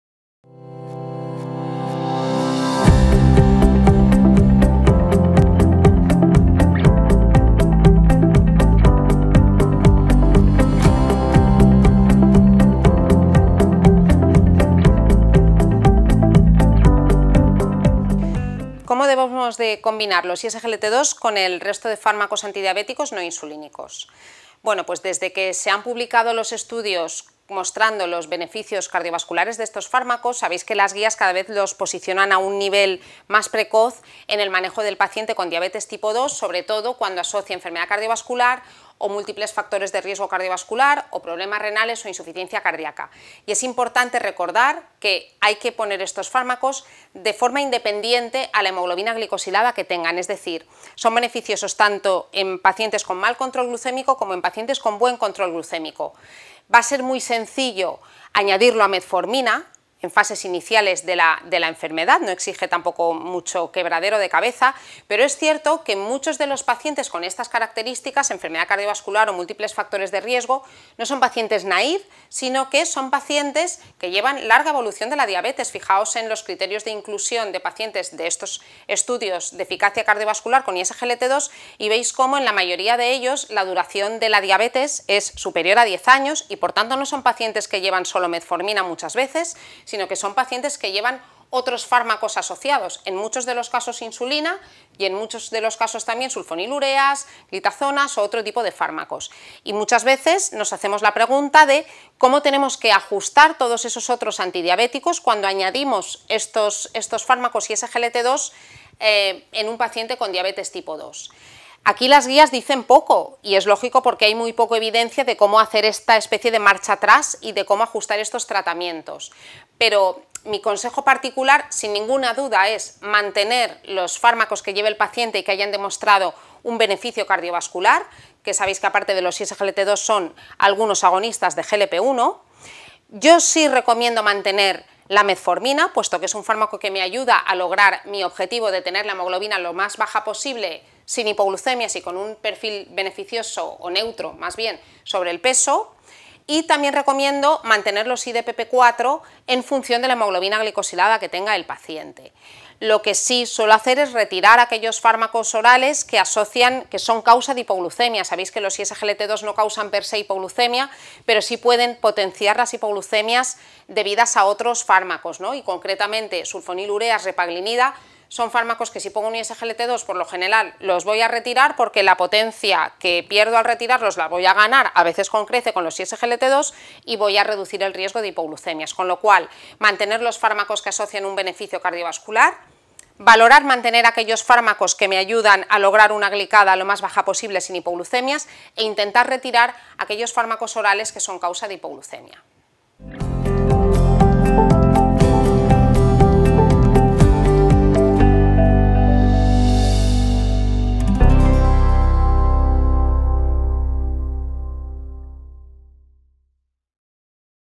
¿Cómo combinar los iSGLT2 con los antidiabéticos no insulínicos?
La Dra Eva Solá nos comenta los "trucos" para poder combinar los iSGLT2 con el resto de fármacos antidiabéticos no insulínicos